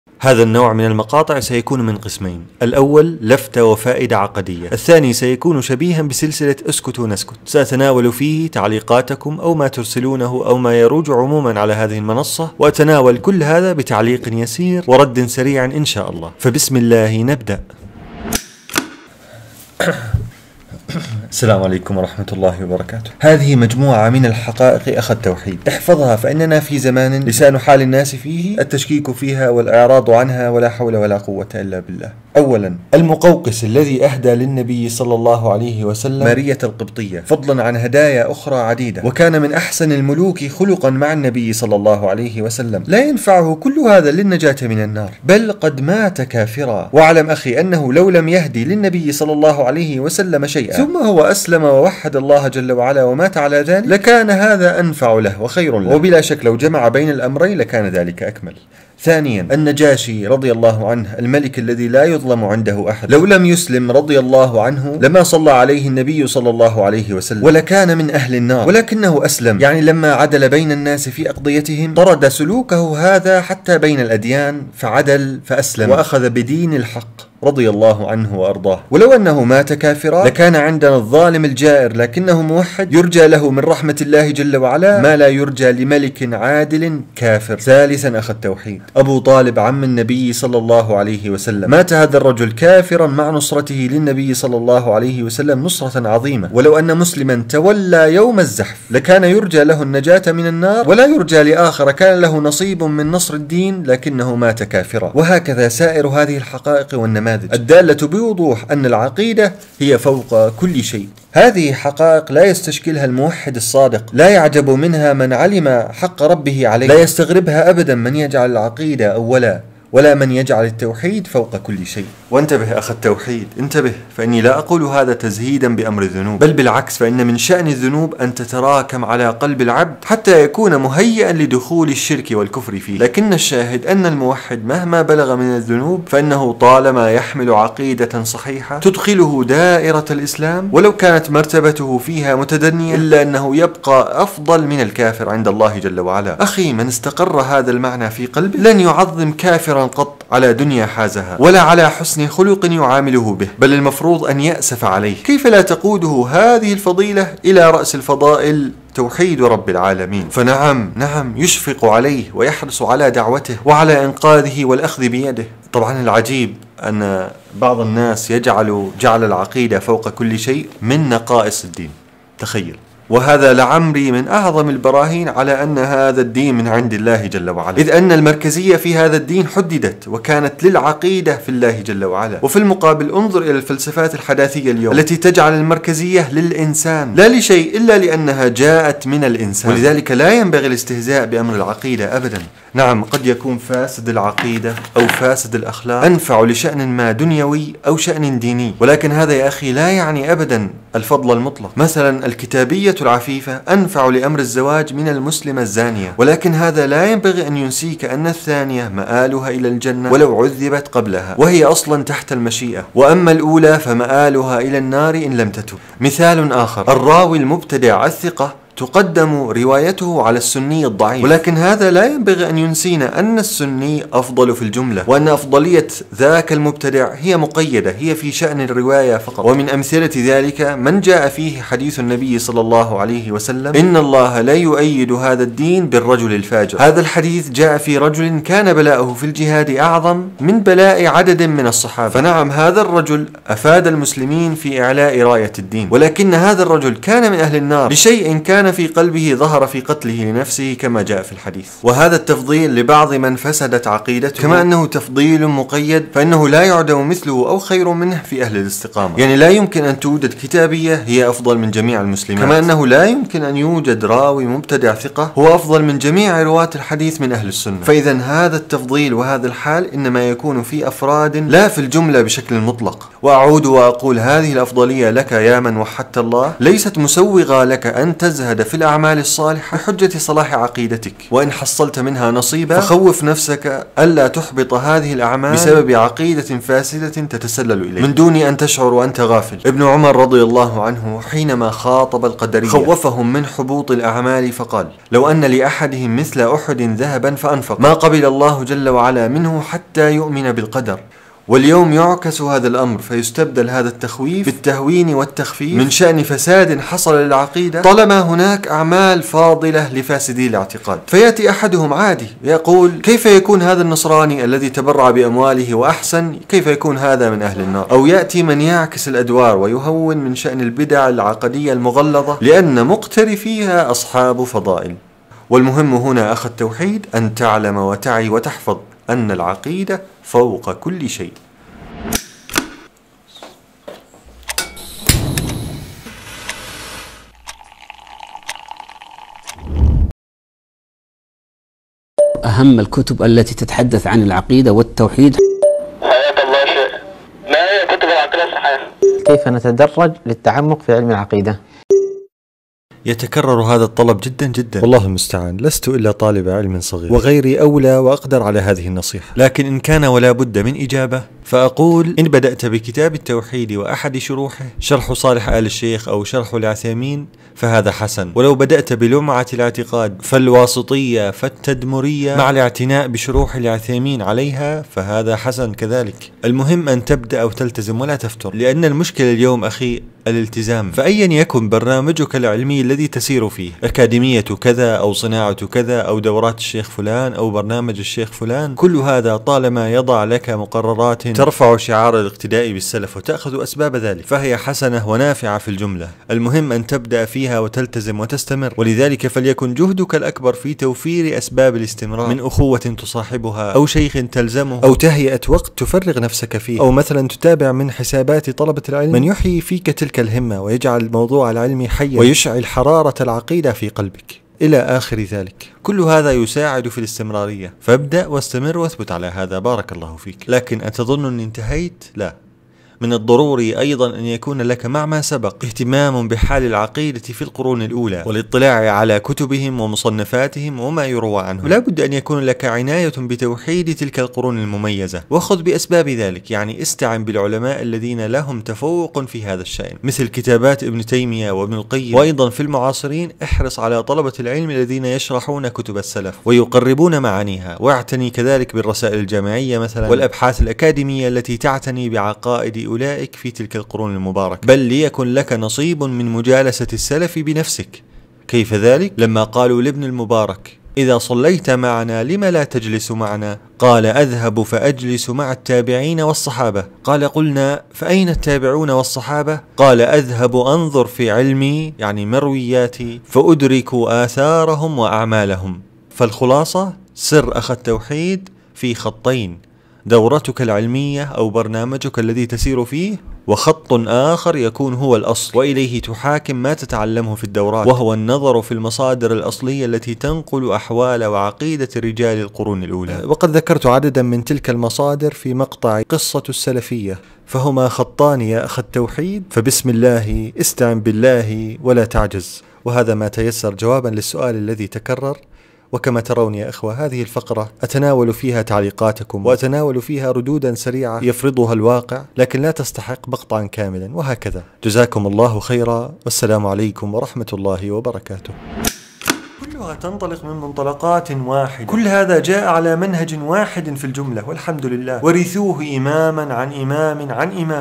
نماذج تؤكد لك بوضوح أن العقيدة فوق كل شيء || إجابة سؤال: كيف أبدأ طلب علم التوحيد؟
☑ روابط الكتب المشار إليها في المقطع

☑ تقديم..
المهندس أبو عبد الرحمن حسام بن علي
طالبُ دراساتٍ عليا في كلية الشريعة في الجامعة الأردنية (سابقًا)

وإذا رغبت أخي في الدعم والرعاية فهو على التفصيل التالي:
✓ دعم عام (تفريغ القائمين على القناة ونحو ذلك)

00:00 المقطع يتكون من قسمين
00:18 ((بداية القسم الأول))
00:32 حال المقوقس ومآله
00:59 حال النجاشي ومآله رضي الله عنه
01:27 حال أبو طالب ومآله
02:00 حال الموحدين مع الذنوب
02:26 أثر العلم باختصاص أهل التوحيد بالنعيم والرضا دون غيرهم
02:41 نظرة الموحد لأهل الكفر والشرك
02:46 هل جعل العقيدة أولًا من سلبيات الدين!
03:14 الإستثناء في التفضيل لبعض فاسدي العقيدة
03:22 أمثلة على التفضيل المقيد والمتعلق بأفراد وحسب
04:33 حال الموحدين مع الطاعات
05:01 حال الكثير اليوم مع الطائعين من فاسدي العقيدة
05:33 ((بداية القسم الثاني))
06:02 الخط الأول
07:00 الخط الثاني
07:56 خلاصة طلب علم الإعتقاد في خطين
08:46 خاتمة